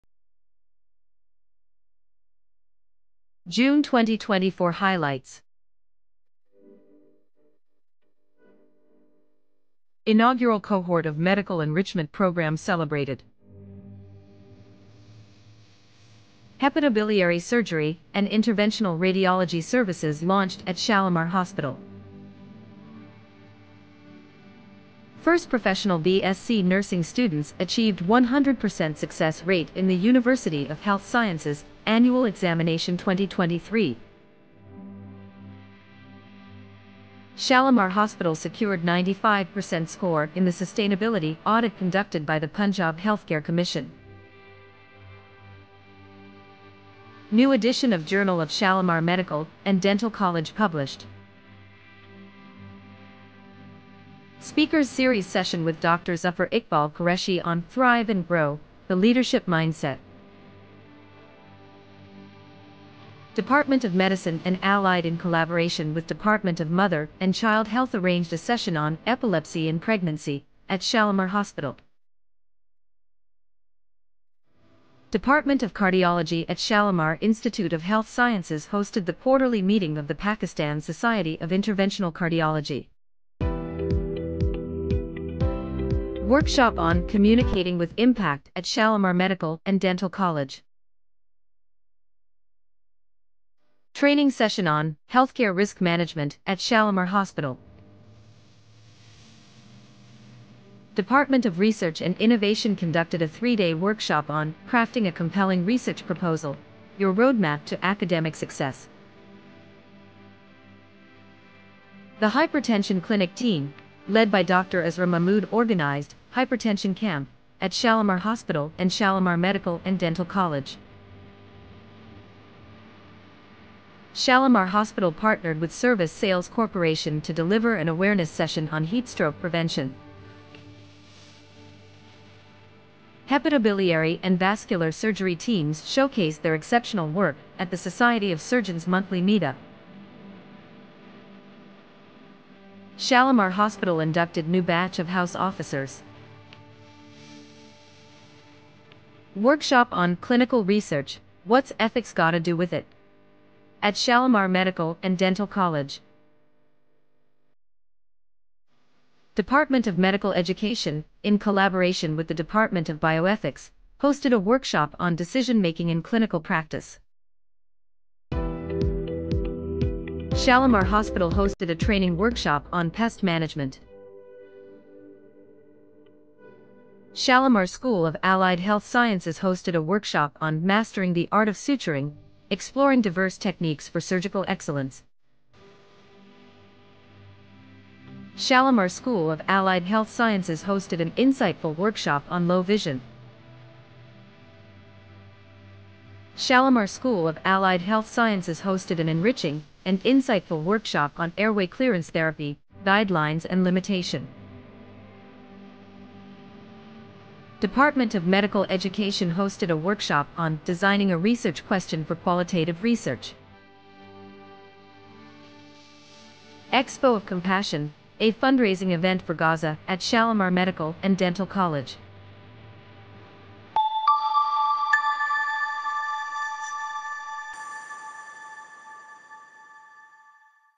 Highlights June 2024
Celebrating milestones and achievements at Shalamar Institute of Health Sciences this June! From new programs and workshops to exceptional success rates and impactful initiatives, our community continues to thrive and make a difference. Watch our monthly highlights to see all that we've accomplished together!